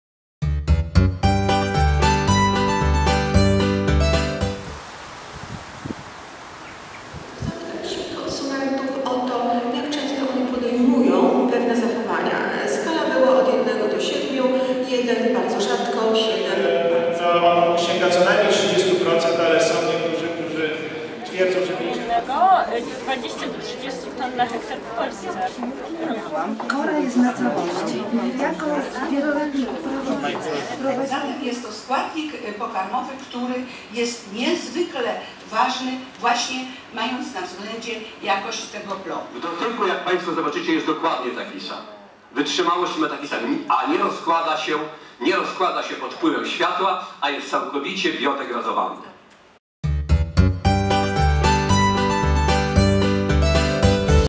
V Zjazd PTNO. Konferencja (17.09.2019)
W Szkole Głównej Gospodarstwa Wiejskiego w Warszawie miała miejsce konferencja naukowa w ramach V Zjazdu Polskiego Towarzystwa Nauk Ogrodniczych. Naukowcy z różnych ośrodków badawczych przedstawiali wyniki swoich badań w formie wykładów i posterów.